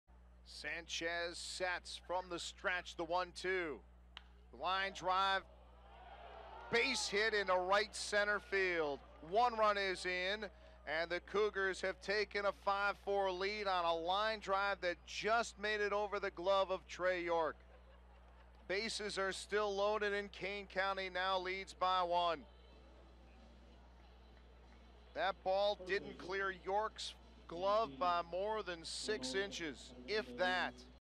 Karaviotis strokes go-ahead single for Kane County
6/11/17: Mark Karaviotis delivers a go-ahead single in the ninth inning of the Cougars' 6-4 win against Wisconsin